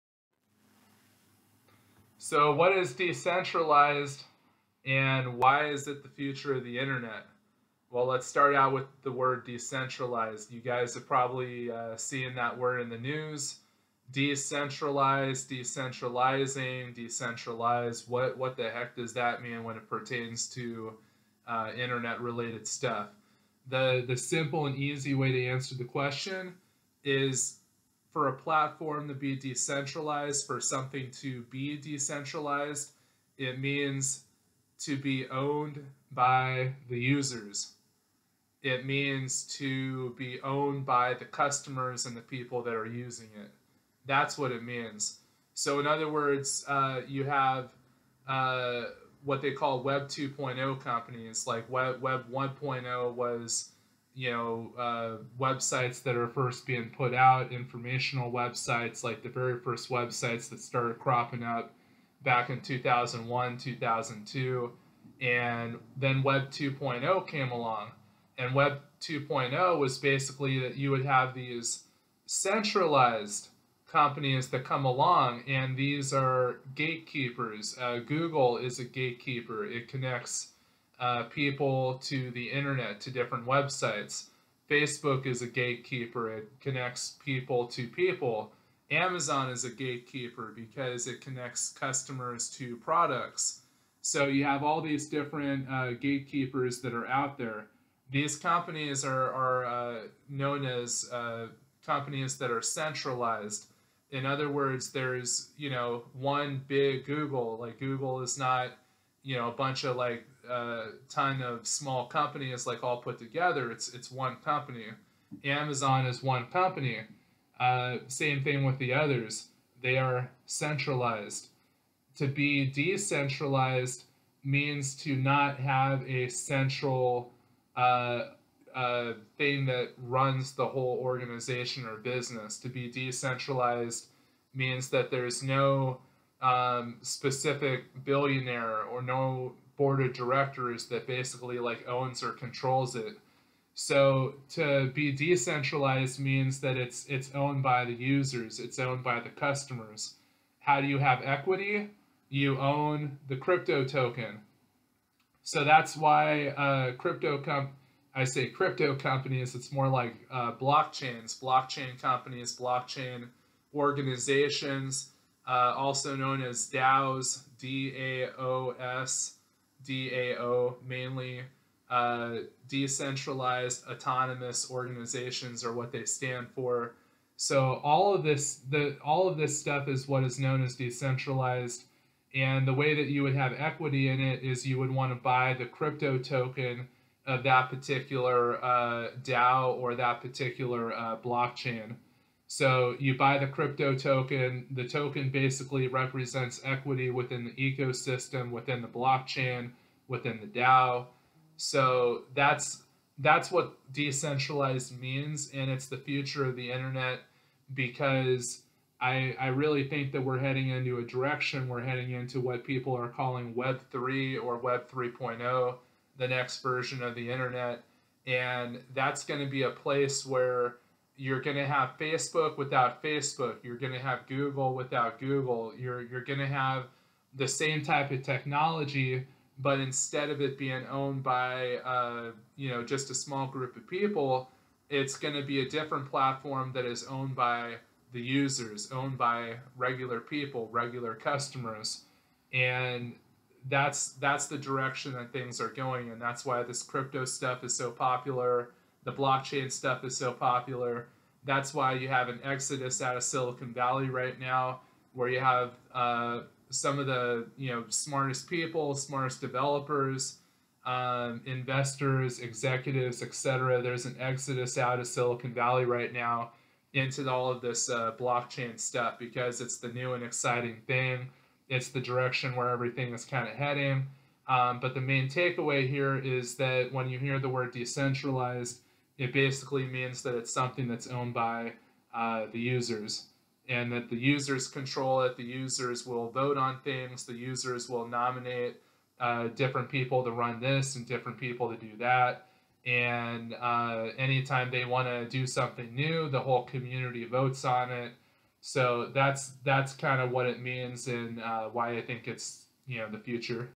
What does decentralized mean? Why is it the future of the internet?
The next version of the internet will remove centralized companies like Google and Facebook. Over the next few years there will be similar platforms that rise up. That connect creators directly with viewers. And connect peer to peer. These platforms will be owned by the users. And each platform's crypto token will be a way for people to own equity.